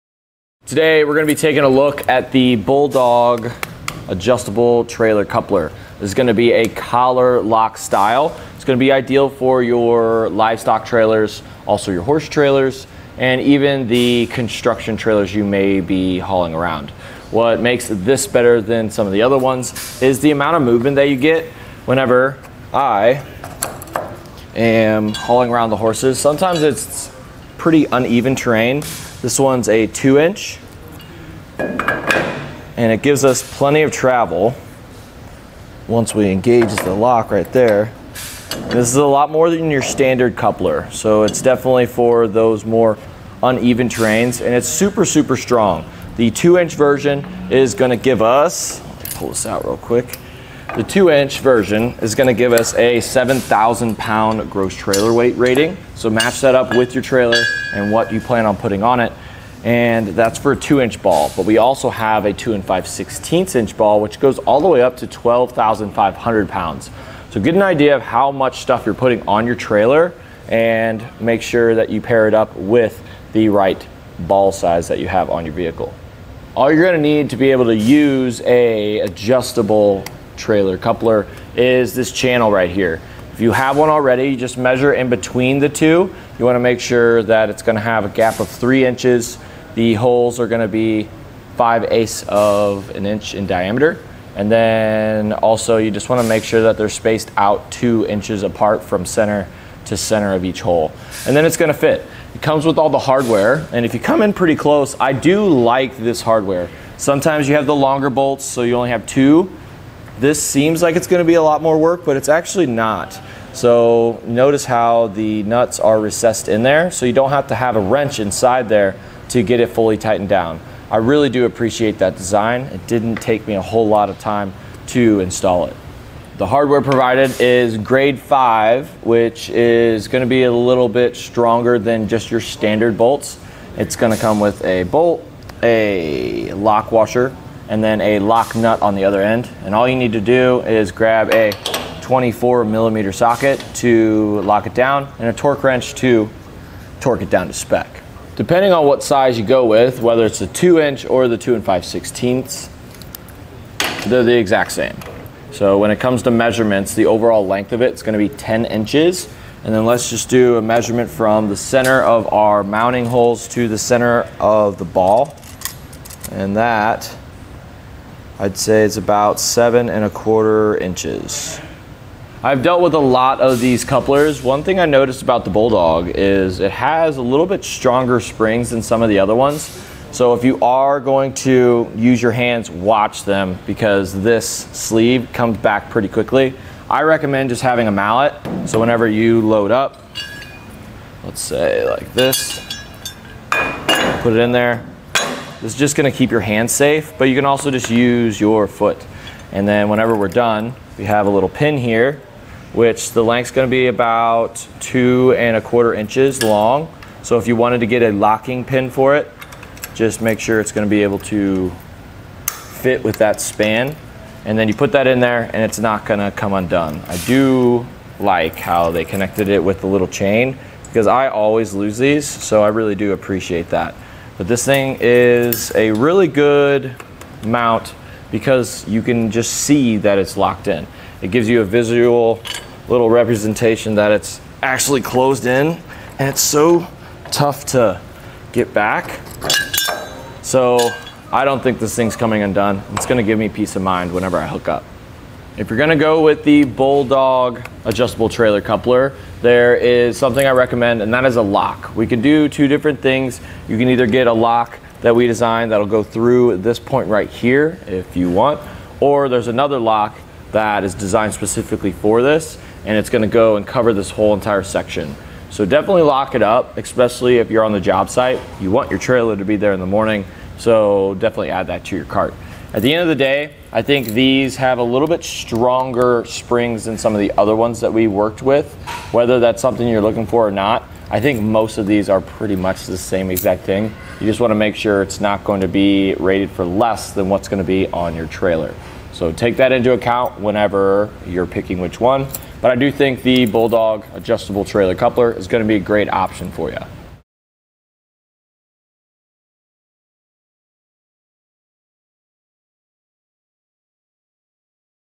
etrailer | Complete Breakdown of the Bulldog Collar-Lok Coupler
Today, we're gonna be taking a look at the Bulldog adjustable trailer coupler. It's gonna be a collar lock style. It's gonna be ideal for your livestock trailers. Also your horse trailers, and even the construction trailers you may be hauling around. What makes this better than some of the other ones is the amount of movement that you get. Whenever I am hauling around the horses, sometimes it's pretty uneven terrain.

This one's a two inch and it gives us plenty of travel, once we engage with the lock right there. This is a lot more than your standard coupler. So it's definitely for those more uneven terrains and it's super, super strong. The two inch version is gonna give us, pull this out real quick. The two inch version is gonna give us a 7,000 pound gross trailer weight rating.

So match that up with your trailer and what you plan on putting on it. And that's for a two inch ball, but we also have a two and five sixteenths inch ball, which goes all the way up to 12,500 pounds. So get an idea of how much stuff you're putting on your trailer and make sure that you pair it up with the right ball size that you have on your vehicle. All you're gonna need to be able to use a adjustable trailer coupler is this channel right here. If you have one already, you just measure in between the two, you wanna make sure that it's gonna have a gap of three inches.

The holes are gonna be five eighths of an inch in diameter. And then also, you just wanna make sure that they're spaced out two inches apart from center to center of each hole, and then it's gonna fit. It comes with all the hardware. And if you come in pretty close, I do like this hardware. Sometimes you have the longer bolts, so you only have two.

This seems like it's gonna be a lot more work, but it's actually not. So notice how the nuts are recessed in there. So you don't have to have a wrench inside there to get it fully tightened down. I really do appreciate that design. It didn't take me a whole lot of time to install it. The hardware provided is grade five, which is gonna be a little bit stronger than just your standard bolts. It's gonna come with a bolt, a lock washer, and then a lock nut on the other end. And all you need to do is grab a 24 millimeter socket to lock it down and a torque wrench to torque it down to spec. Depending on what size you go with, whether it's a two inch or the two and five sixteenths, they're the exact same. So when it comes to measurements, the overall length of it's gonna be 10 inches. And then let's just do a measurement from the center of our mounting holes to the center of the ball. And that, I'd say, is about seven and a quarter inches. I've dealt with a lot of these couplers. One thing I noticed about the Bulldog is it has a little bit stronger springs than some of the other ones. So if you are going to use your hands, watch them, because this sleeve comes back pretty quickly. I recommend just having a mallet. So whenever you load up, let's say like this, put it in there, this is just gonna keep your hands safe, but you can also just use your foot. And then whenever we're done, you have a little pin here, which the length's gonna be about two and a quarter inches long. So if you wanted to get a locking pin for it, just make sure it's gonna be able to fit with that span. And then you put that in there and it's not gonna come undone. I do like how they connected it with the little chain 'cause I always lose these, so I really do appreciate that. But this thing is a really good amount because you can just see that it's locked in. It gives you a visual little representation that it's actually closed in and it's so tough to get back. So, I don't think this thing's coming undone. It's gonna give me peace of mind whenever I hook up. If you're gonna go with the Bulldog adjustable trailer coupler, there is something I recommend and that is a lock. We can do two different things. You can either get a lock that we designed that'll go through at this point right here if you want, or there's another lock that is designed specifically for this, and it's gonna go and cover this whole entire section. So definitely lock it up, especially if you're on the job site, you want your trailer to be there in the morning. So, definitely add that to your cart. At the end of the day, I think these have a little bit stronger springs than some of the other ones that we worked with, whether that's something you're looking for or not. I think most of